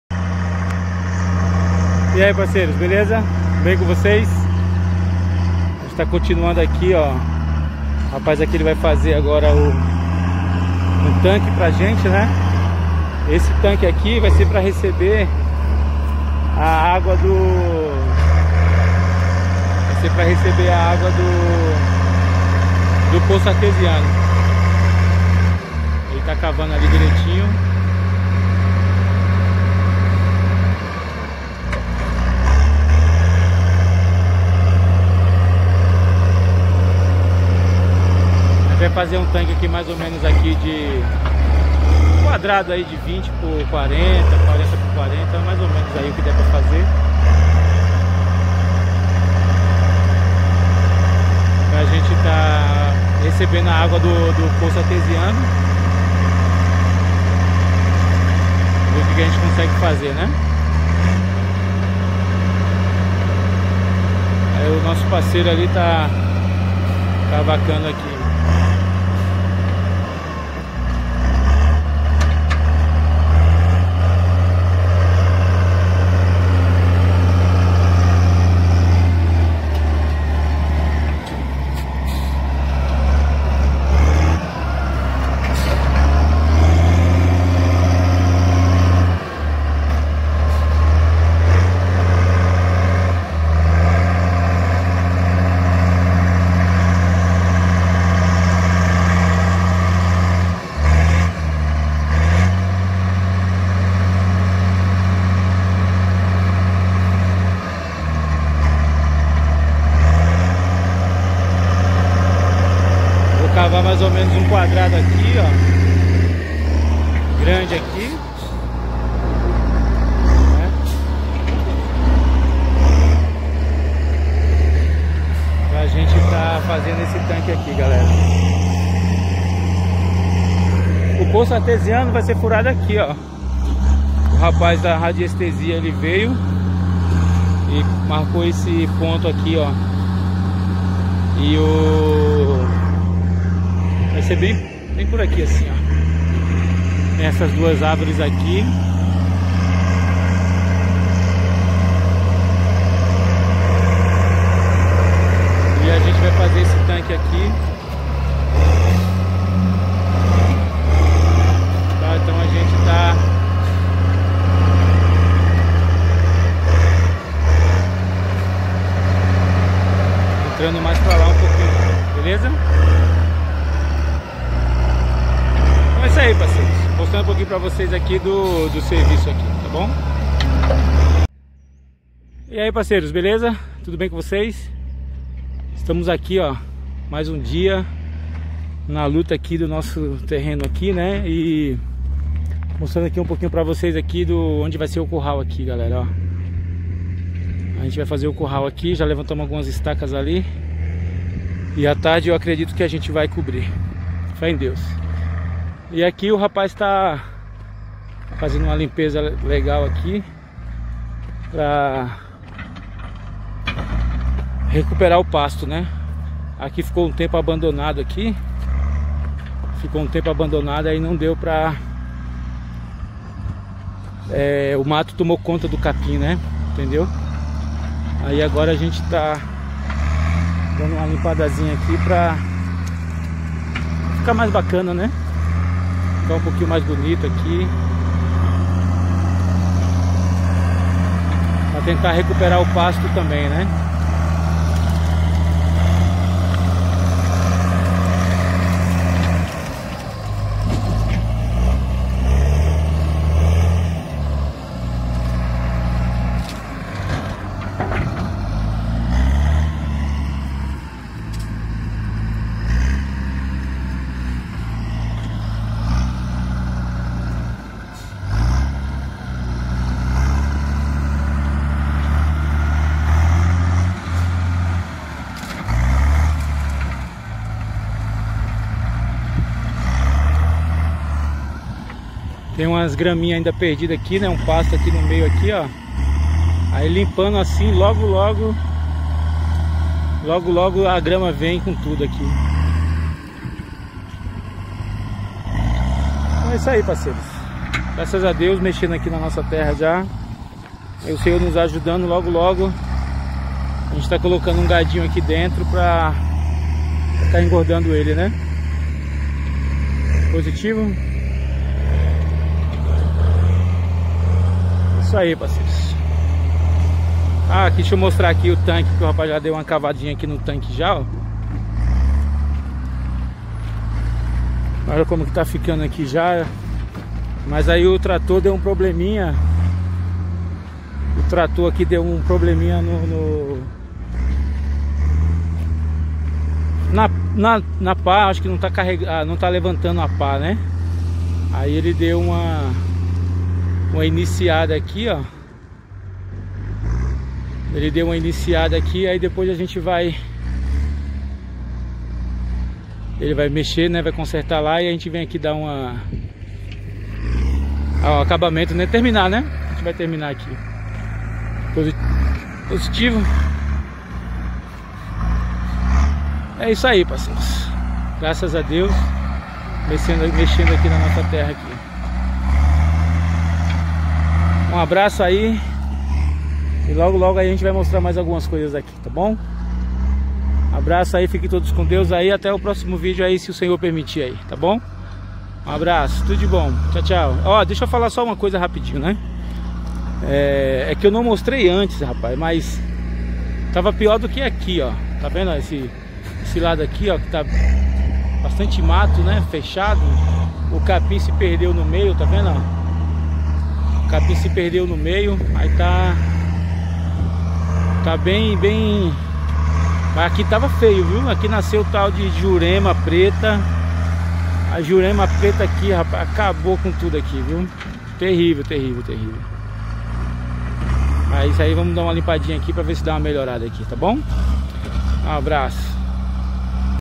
Limpando o terreno e iniciando um tanque.
O nosso futuro lago , e dando uma ageitada no terreno.